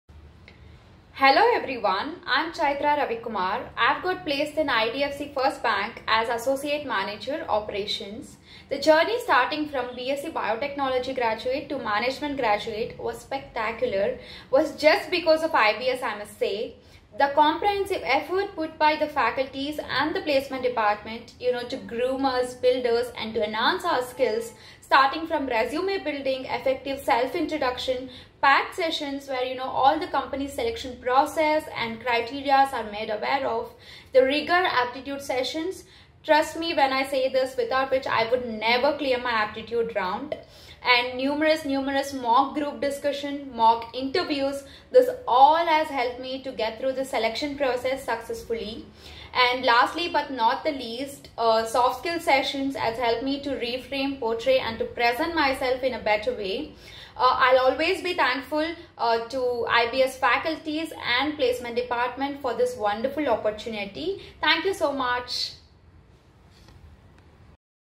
Congratulations Ms. Chaithra Ravikumar placed at IDFC First Bank - as a " Associate Manager "
Congratulations Ms. Chaithra Ravikumar Batch of 2021-23 placed at IDFC First Bank - as a "Associate Manager" | IBS Bangalore Placements